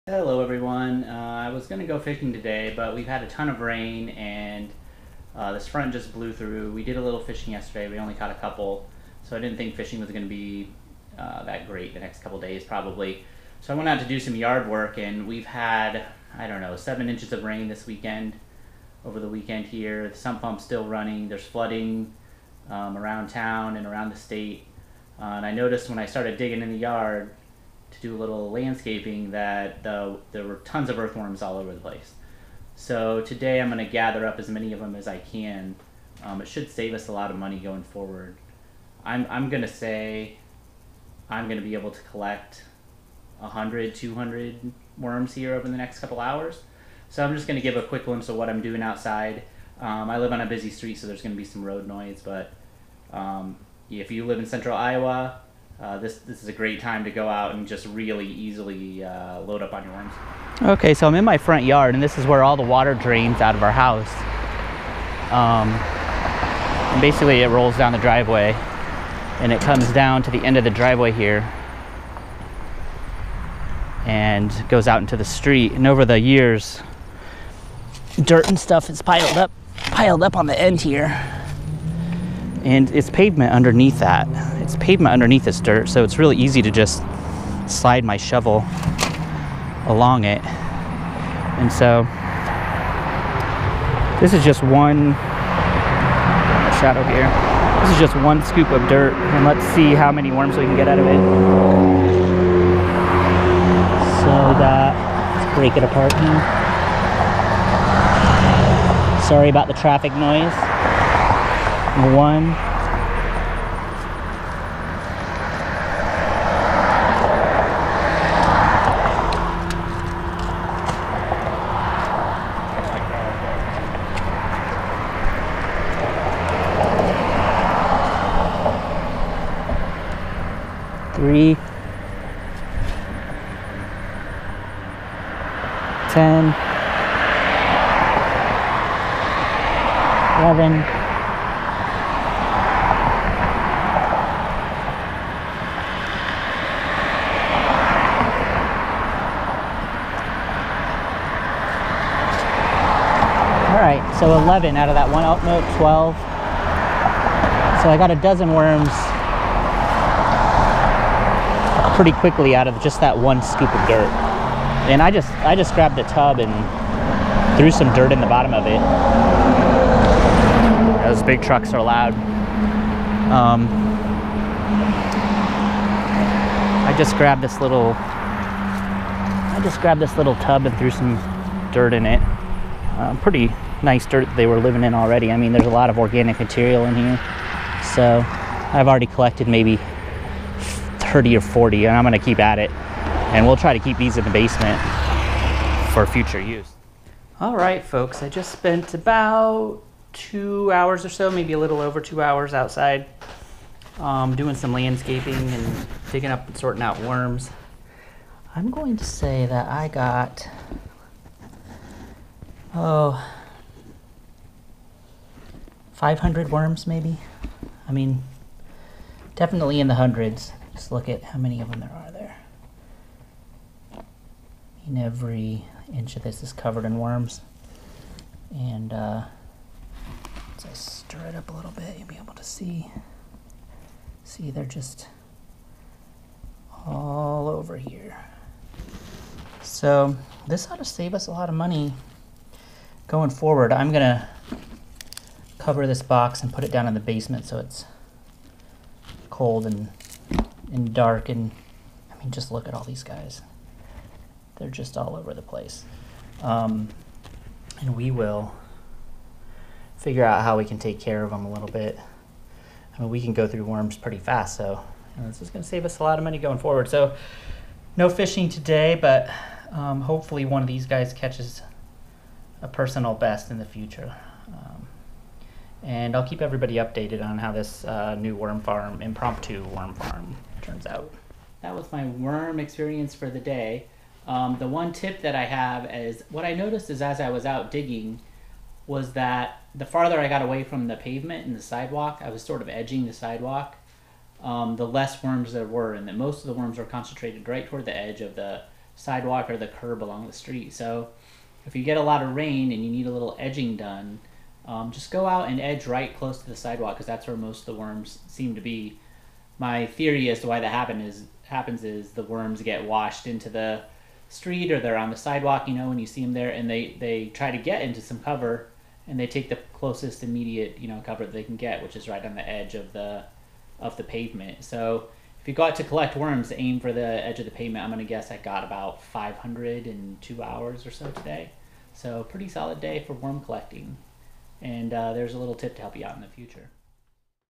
Collecting Fishing Worms: Hundreds in Just 2 Hours
After several inches of rain, I decided to work in the yard instead of fish. I soon noticed it was a perfect day to collect fishing worms.